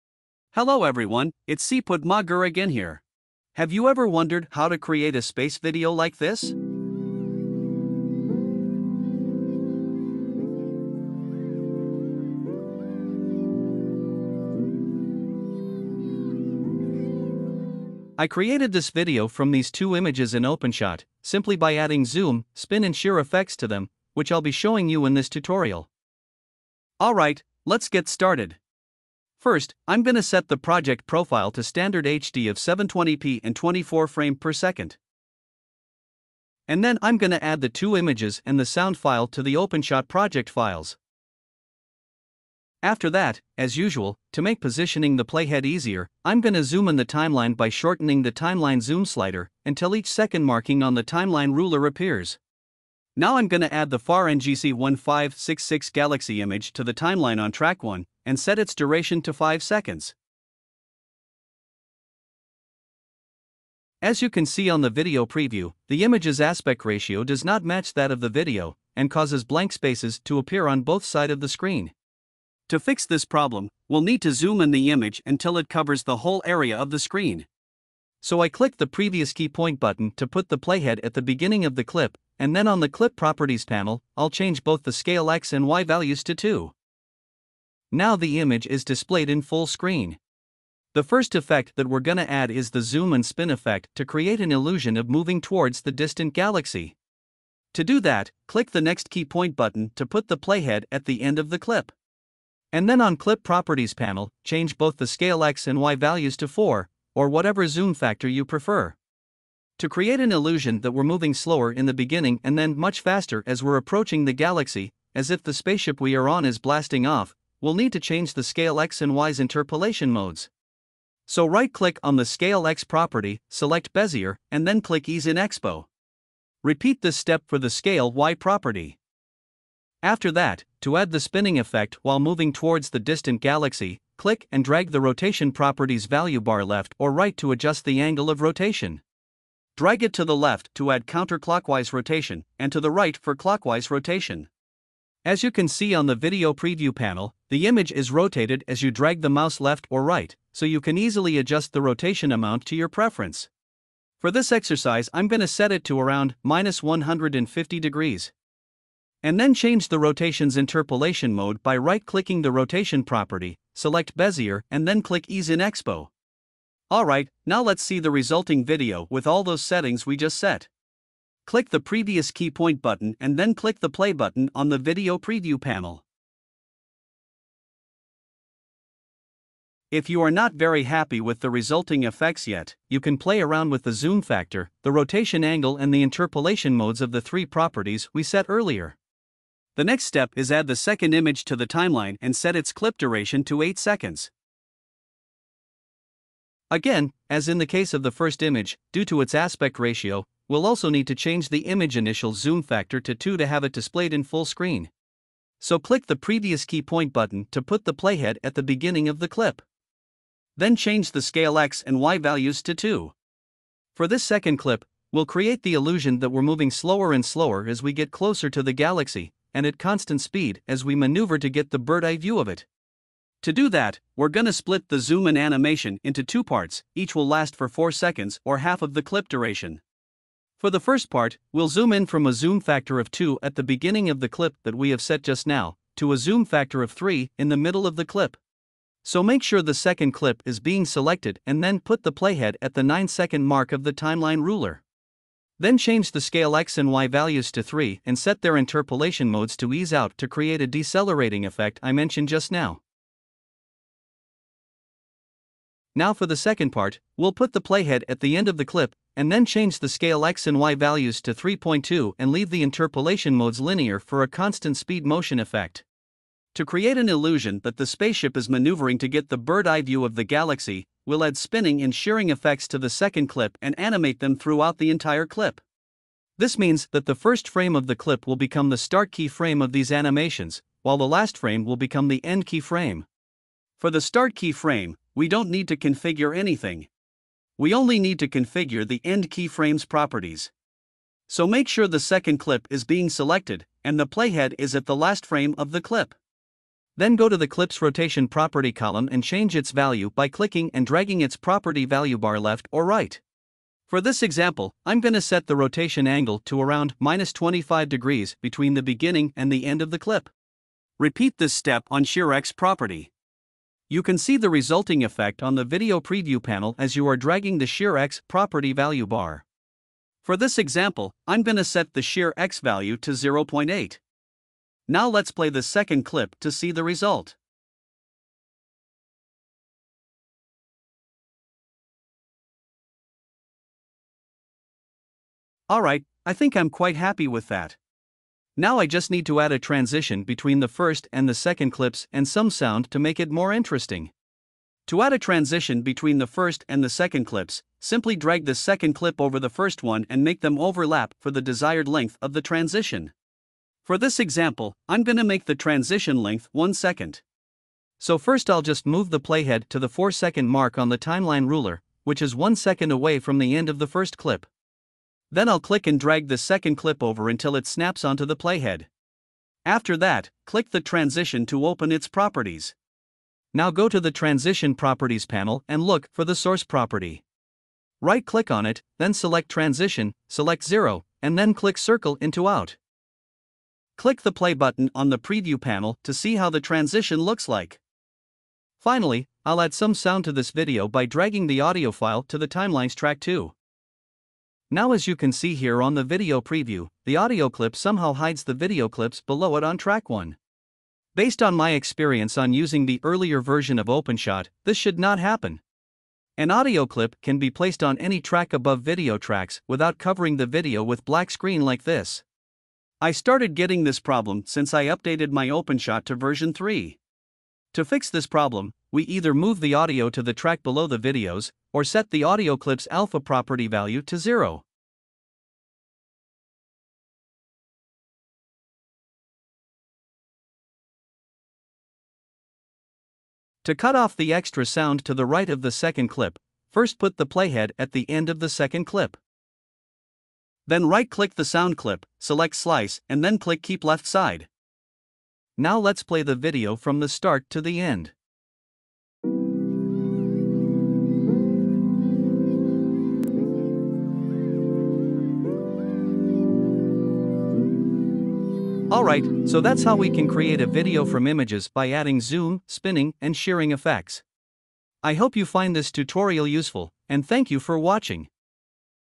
OpenShot Tutorial #3 | How To Create A Space Travel Video From Still Images
OpenShot is an award winning, open source, FREE video editor.

This tutorial shows a complete step-by-step guide on how to create a space travel video from high resolution images by adding zoom, spin and shear effects.

01:30 - Adding zoom effect
02:09 - Adding spin effect
04:56 - Adding spin and shear effects
05:44 - Adding transition

How To Create A Vertical Video From A Widescreen Video For YouTube Short And IG Reel In OpenShot.